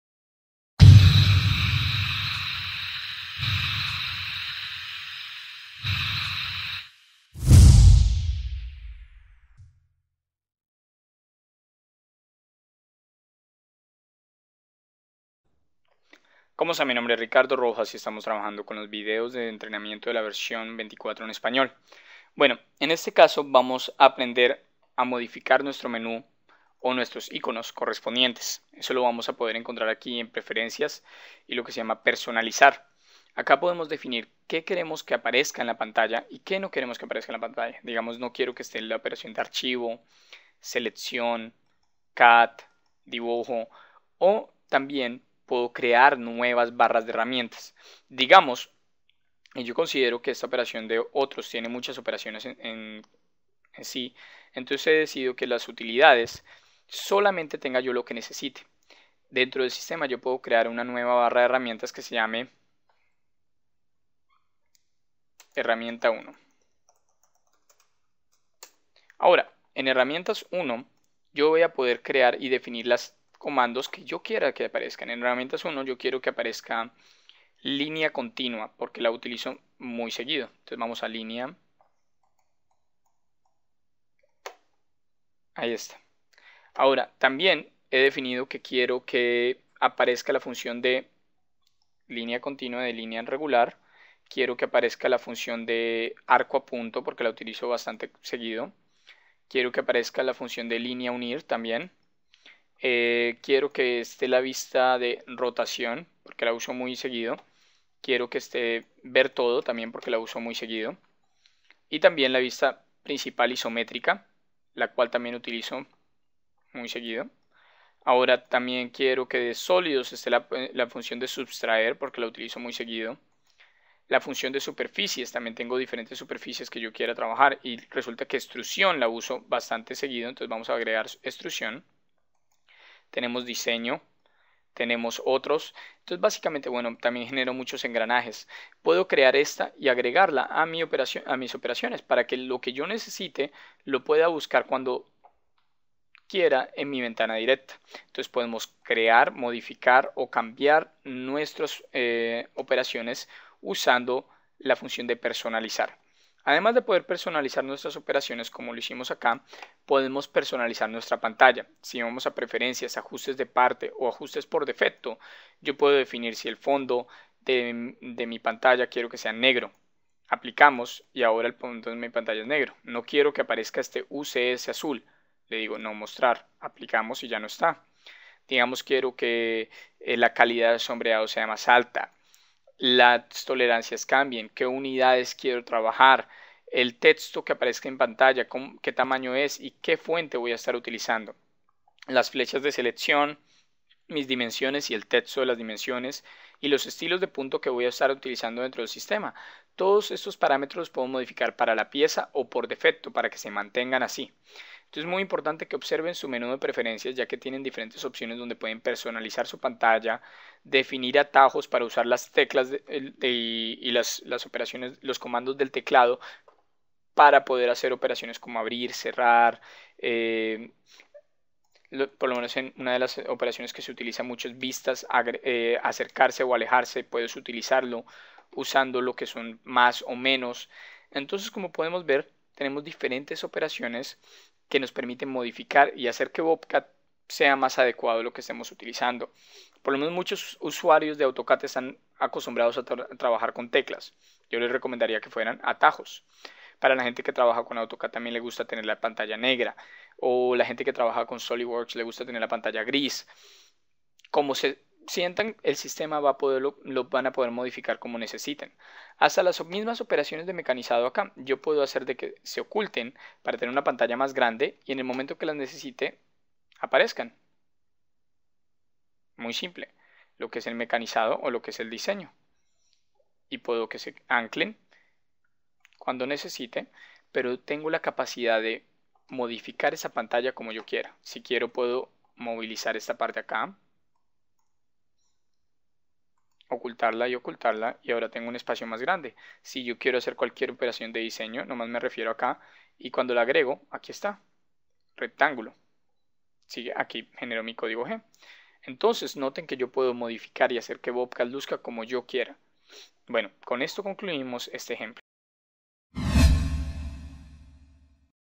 Como Modificar la Interface BobCAD-CAM V24
BobCAD-CAM es un sistema versátil, fácil de utilizar y de la misma manera de modificar. Muchos usuarios están acostumbrados a una interface de uso diferente a la predeterminada por BobCADCAM. Este video demuestra como modificar la interface al gusto del usuario.